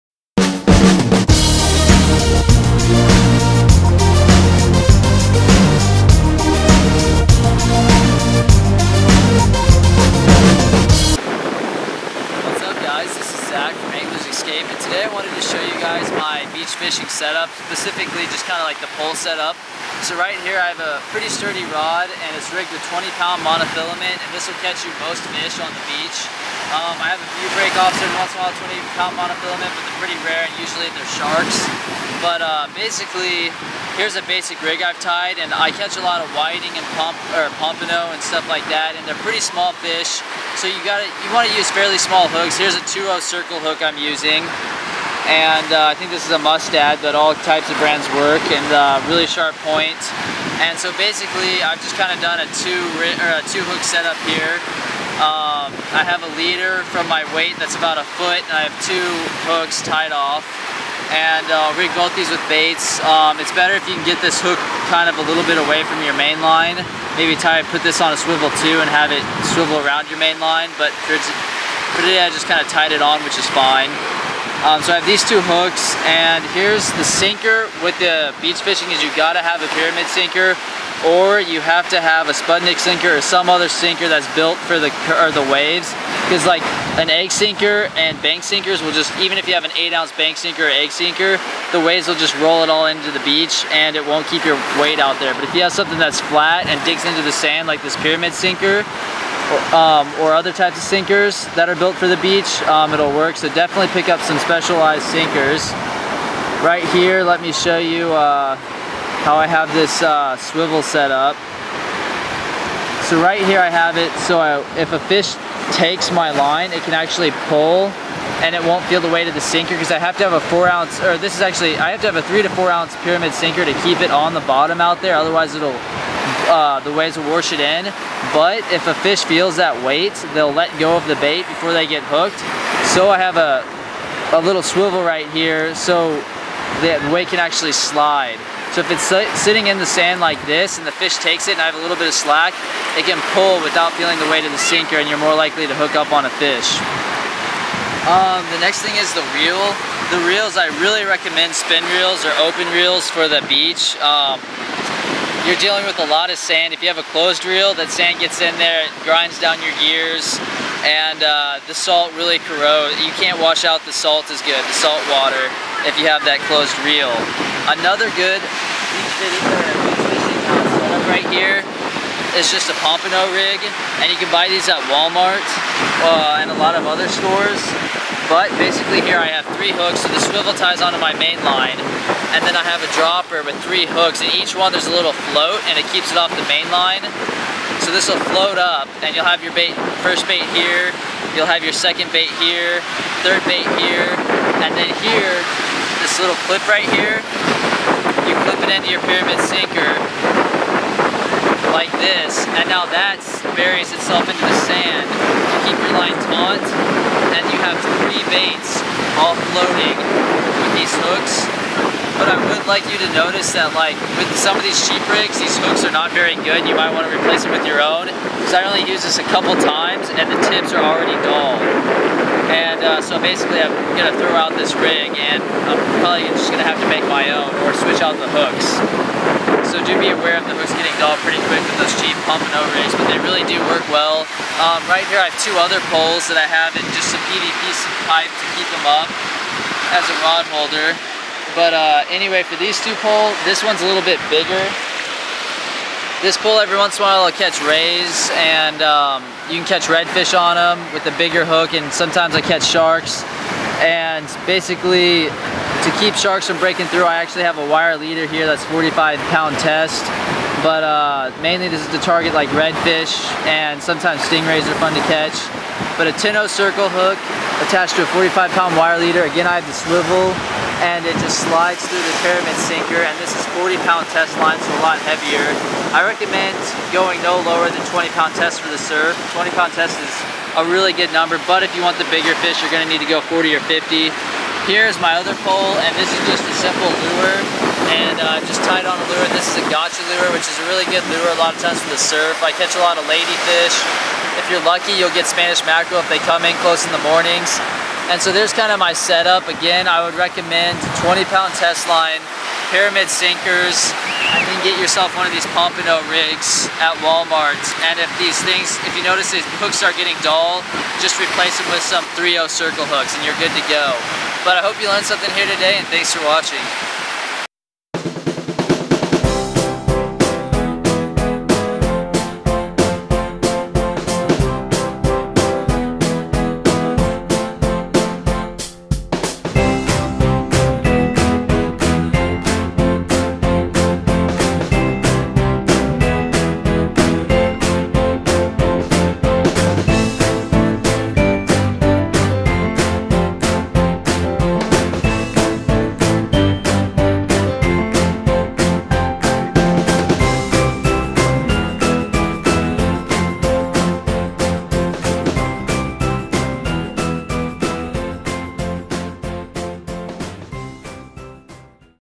Beach Fishing General Tips: Pole, line, sinkers, rod holder, hooks, lures
Here's some beach fishing tips I learned from experience and talking to locals. Hope these beach fishing tips help increase your catch!